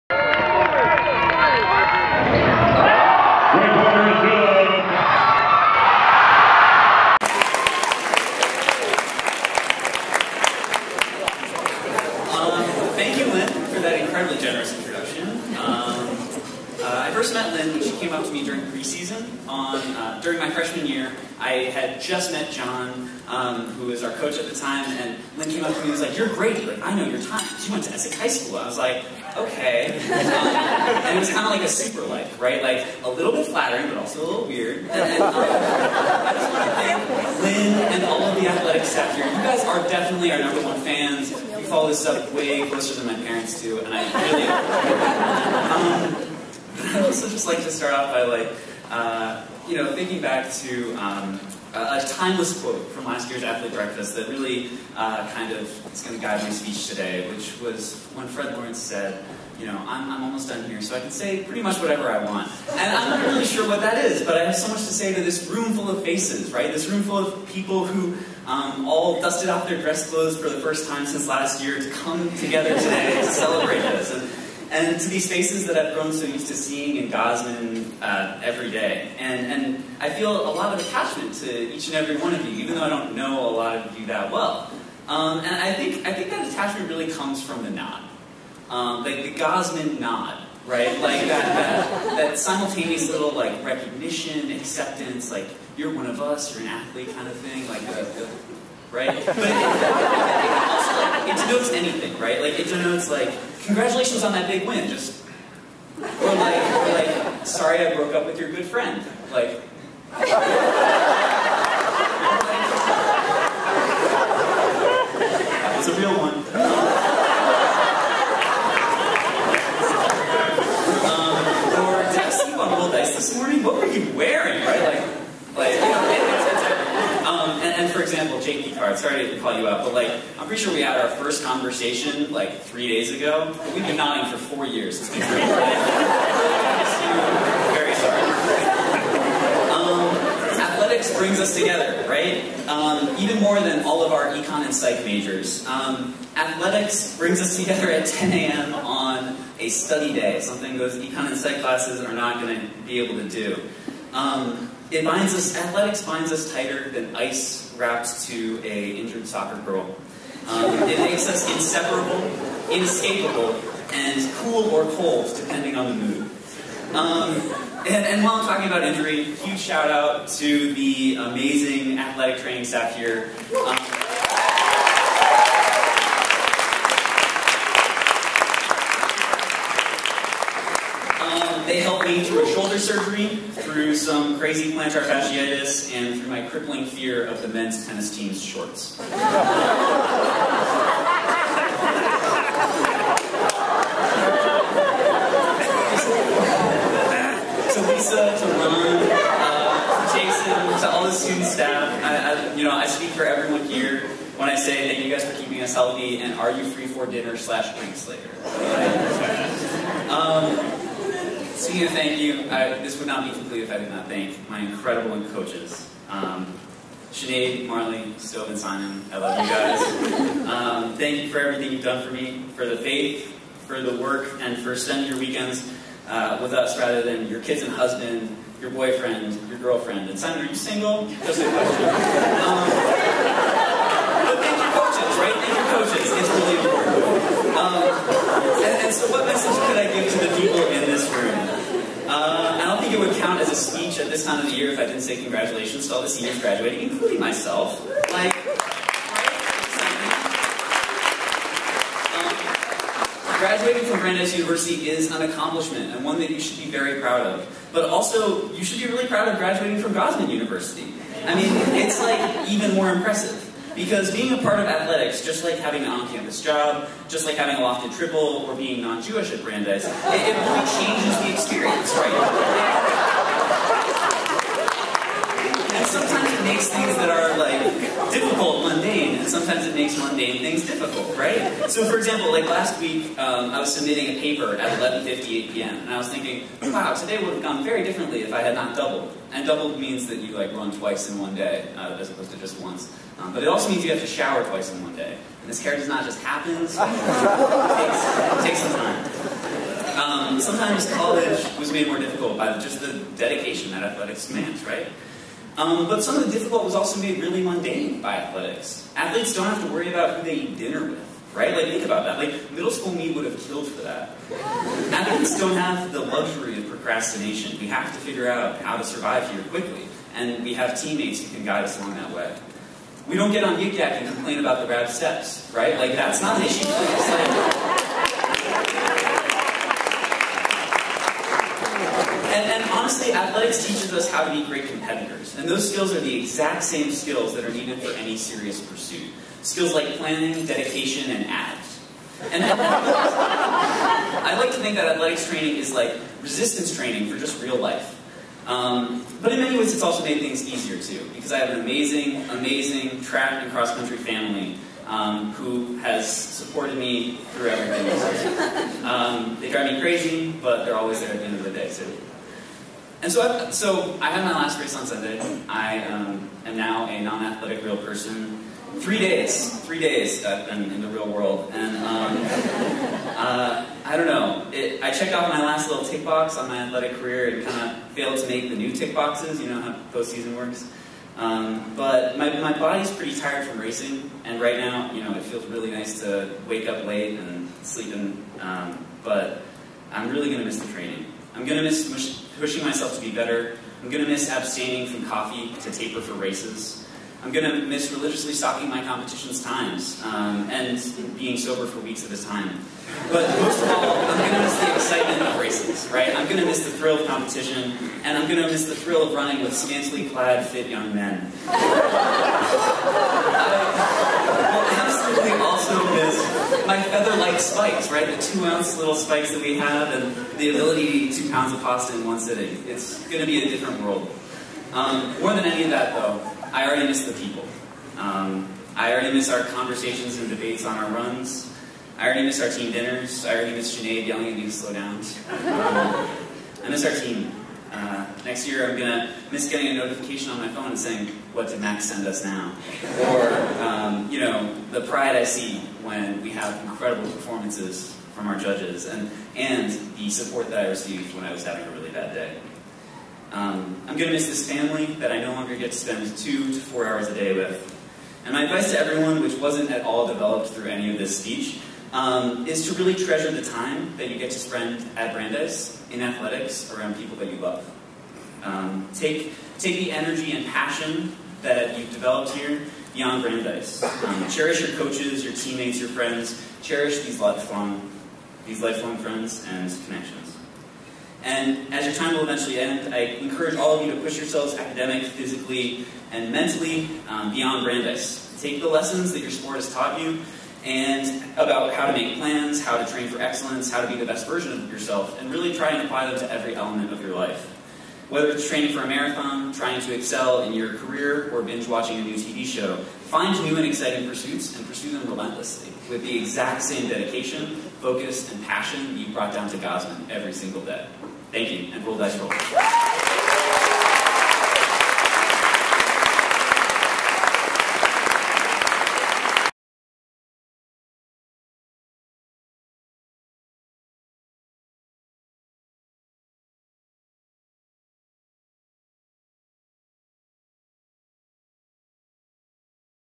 Senior Speech 2016 - Grady Ward
Grady Ward of men's cross country and track delivers one of two Senior Addresses at the end of the year Award Brunch on May 4, 2016.